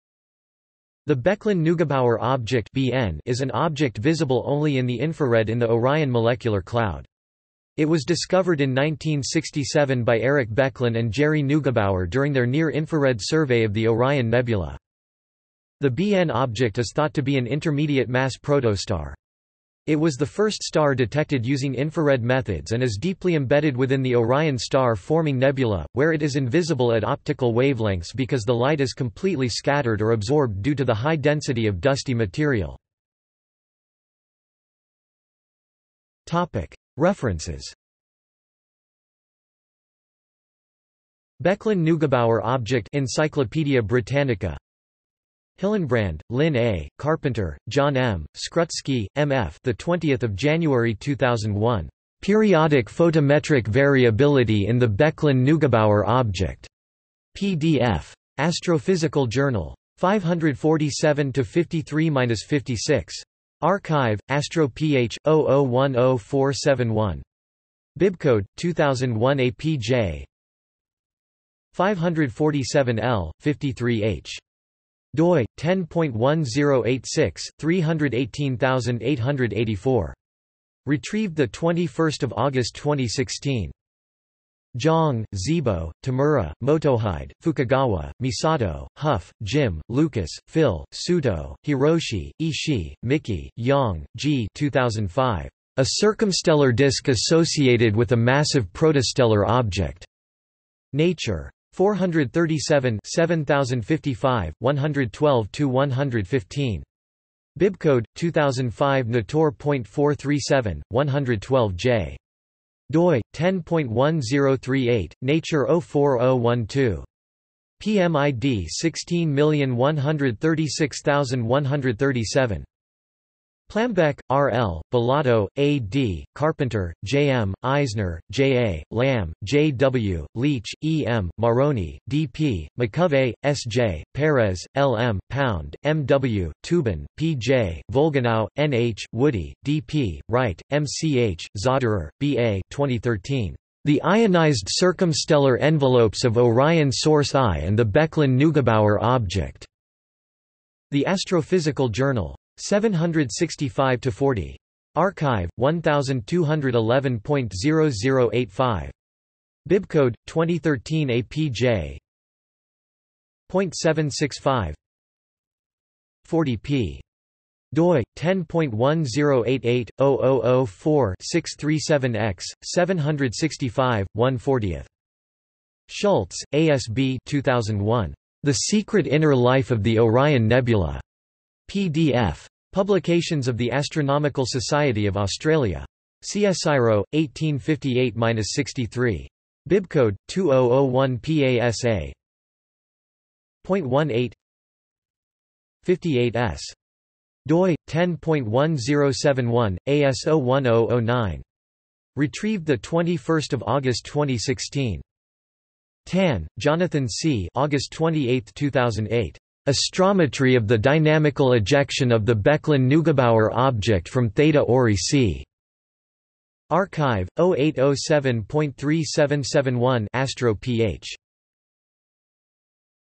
Becklin–Neugebauer Object | Wikipedia audio article
SUMMARY
The Becklin–Neugebauer Object (BN) is an object visible only in the infrared in the Orion Molecular Cloud. It was discovered in 1967 by Eric Becklin and Gerry Neugebauer during their near-infrared survey of the Orion Nebula.
The BN Object is thought to be an intermediate-mass protostar. It was the first star detected using infrared methods and is deeply embedded within the Orion star-forming nebula, where it is invisible at optical wavelengths because the light is completely scattered or absorbed due to the high density of dusty material.